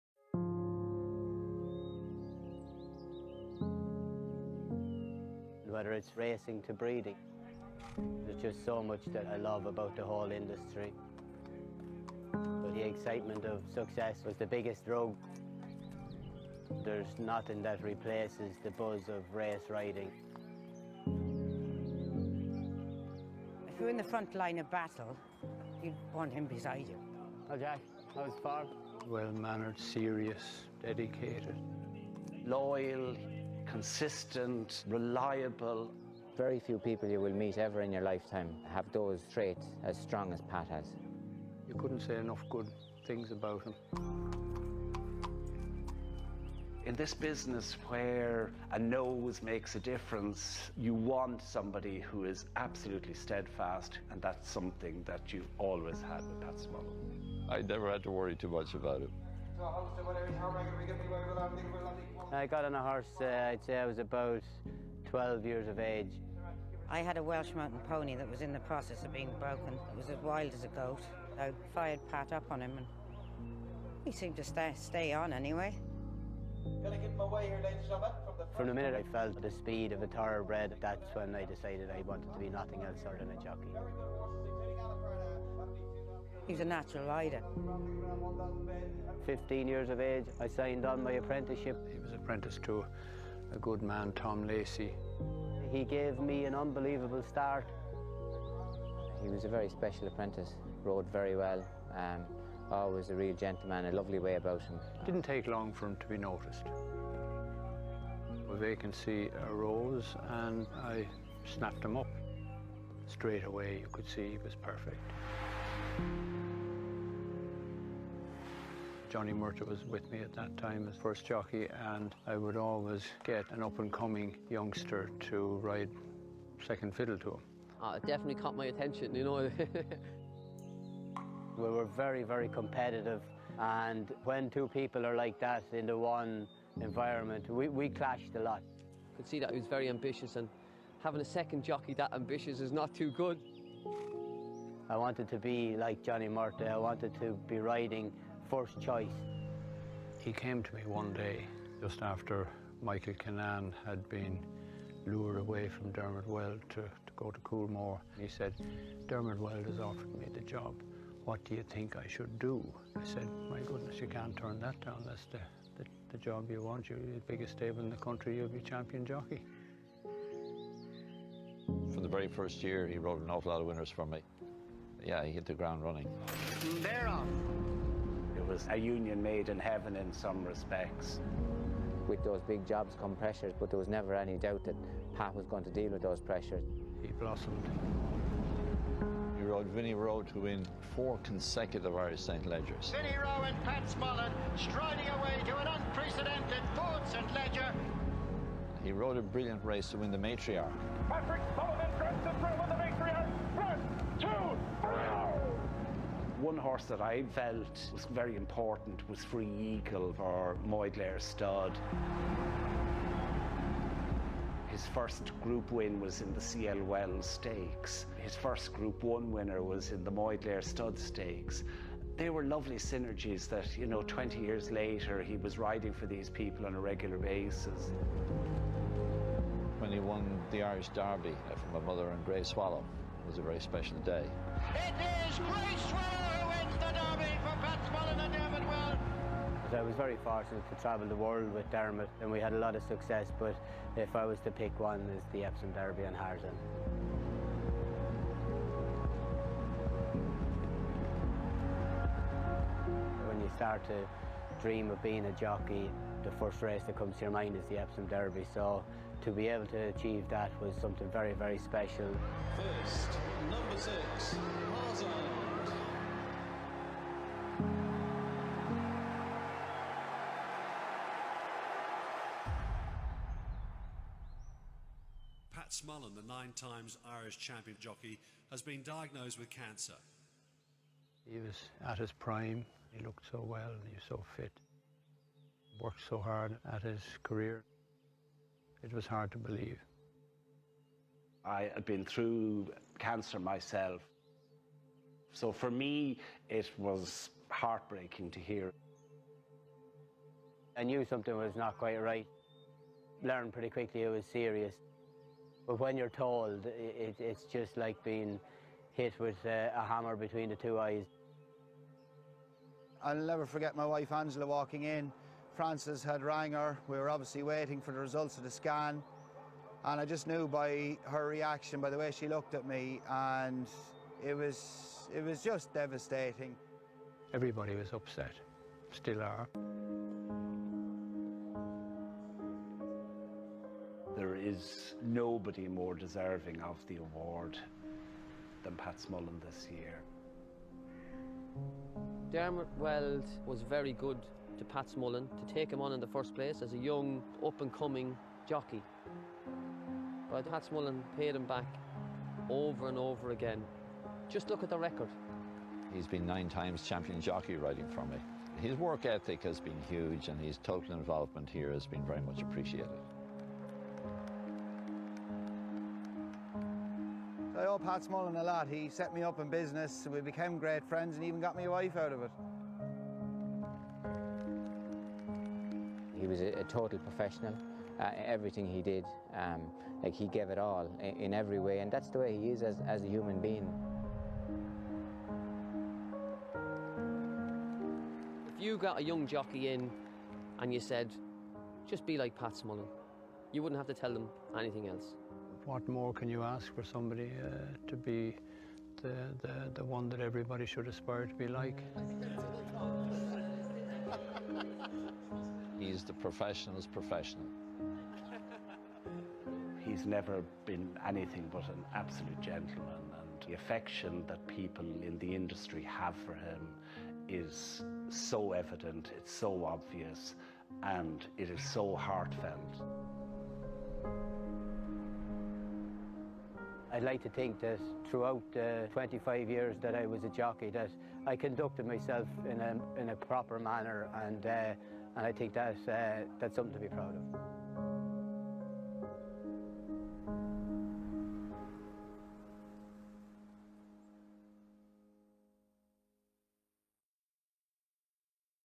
Legend - RIP Pat Smullen
On the devastating news of Pat Smullen's passing, we remember the brilliant jockey who wasn't just a great man in the saddle, but a great man out of it too. RIP Pat...